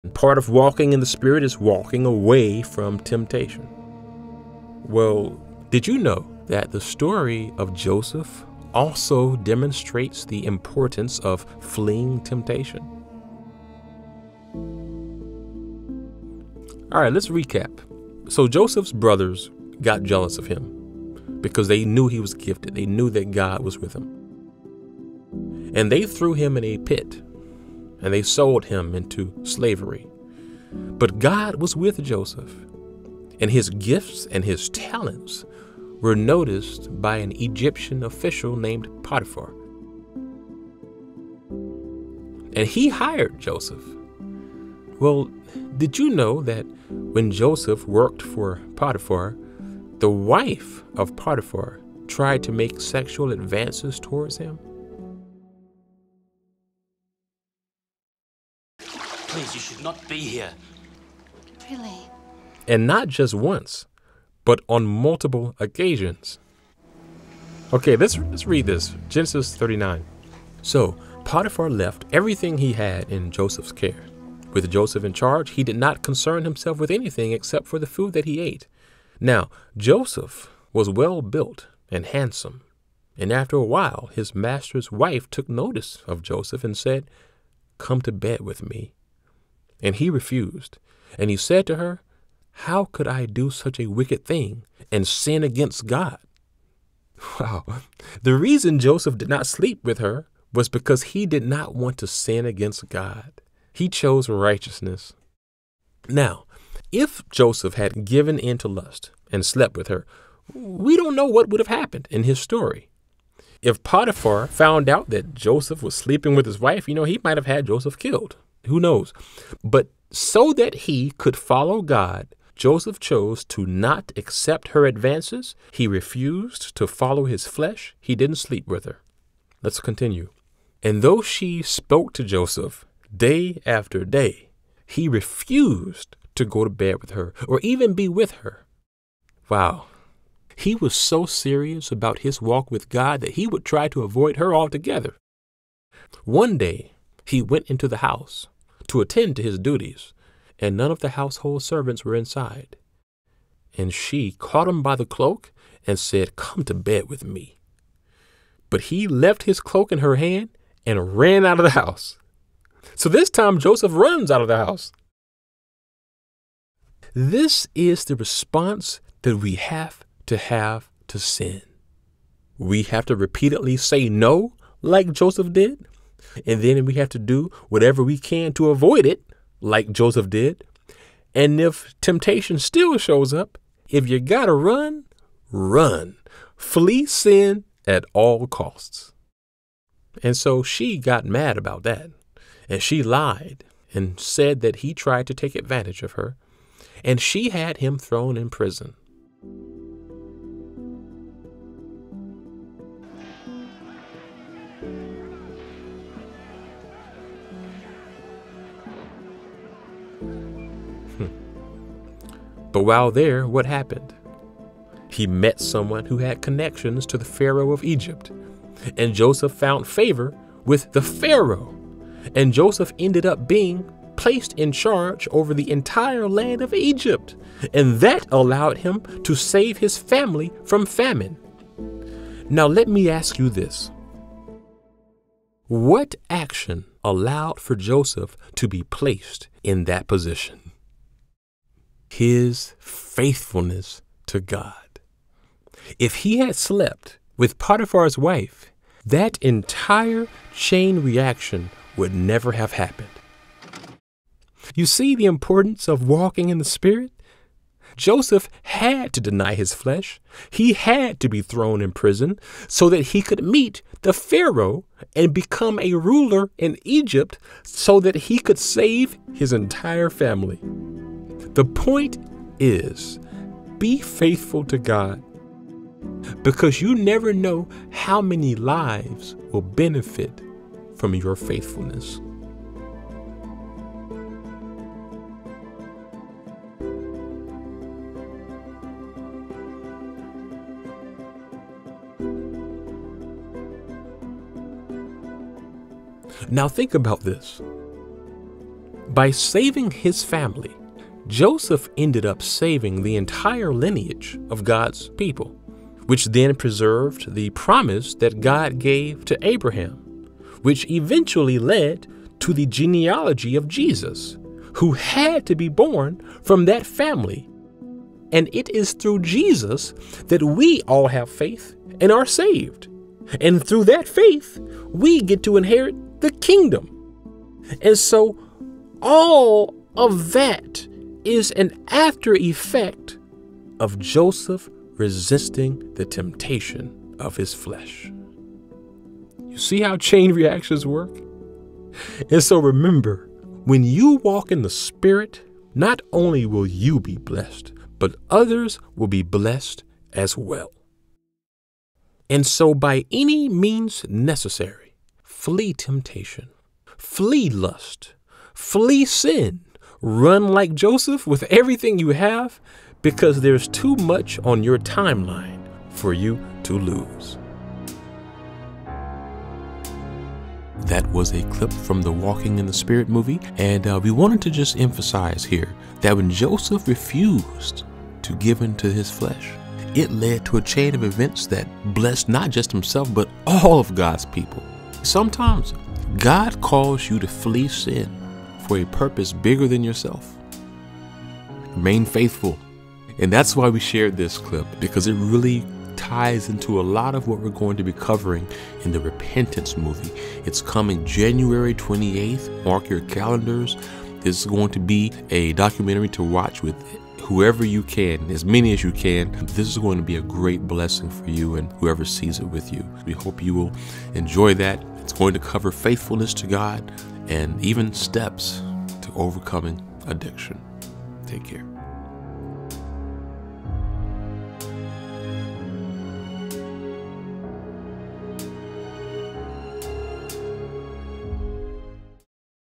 Joseph Resisted Sin and THIS Happened! // Faithfulness to God
When Joseph refused to fornicate, it set off a chain of events that ultimately blessed him and ALL of God’s people! Sometimes God calls you to flee sin for a purpose bigger than just you.

Stay turned for The Repentance Movie coming this month.

Subjects: Joseph and his brothers, Joseph King of Dreams, Bible stories

AoC Network is a Christ-Centered content creator with a goal to represent God’s Kingdom online.